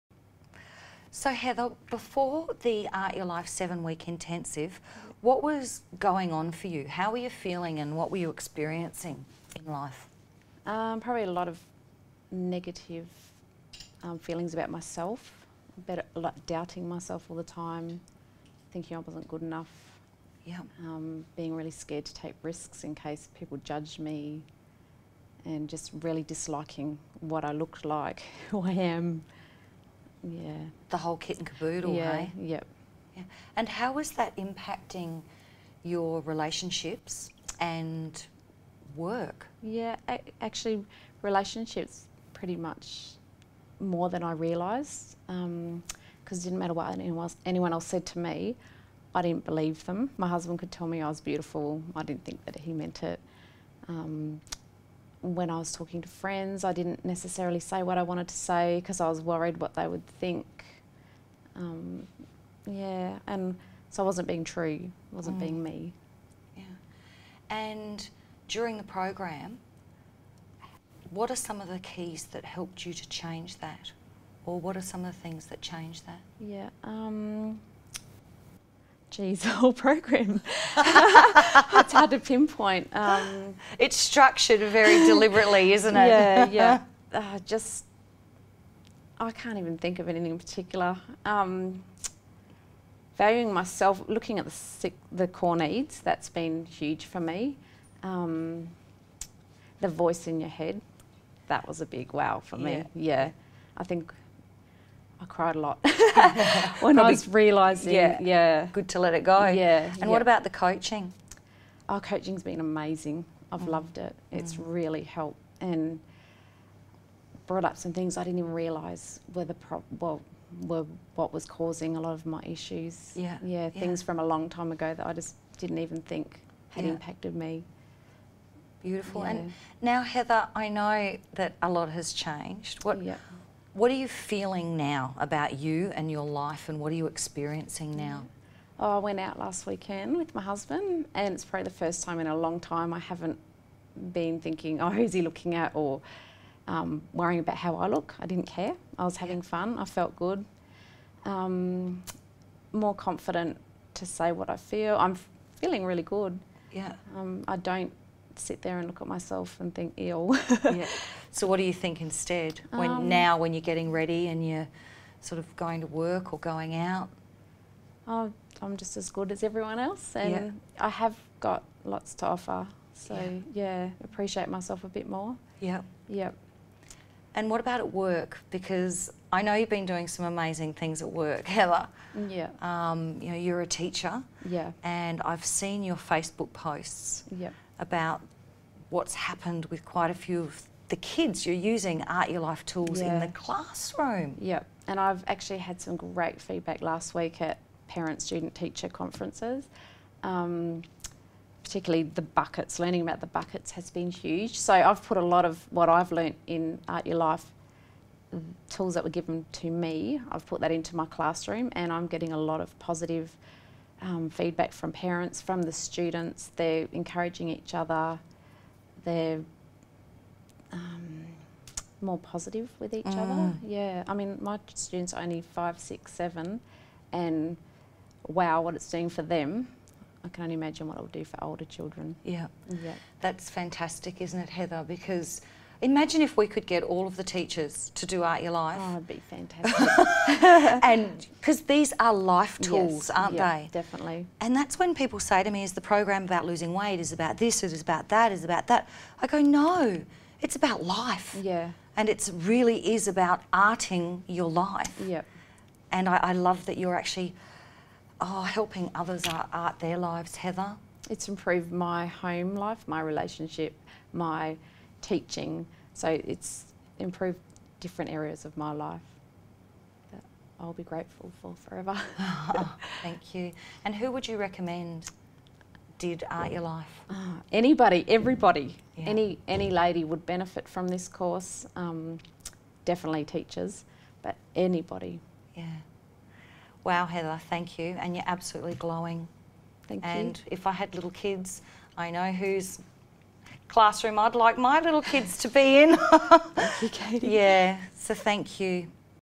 Heather Banks Testimonial on the 7 Week ART Your Life Intensive
Heather shares of story of how she has gone from feeling a lot of negativity about herself, and really disliking what she looked like and who she was - to now valuing herself, feeling more confident and comfortable, and appreciating herself a lot more.

"It has improved my home life, my relationship, my teaching, so it's improved different areas of my life that I'll be grateful for forever."

Heather is a teacher and is using ART Your Life tools in her classroom and is receiving amazing, positive feedback from students and parents.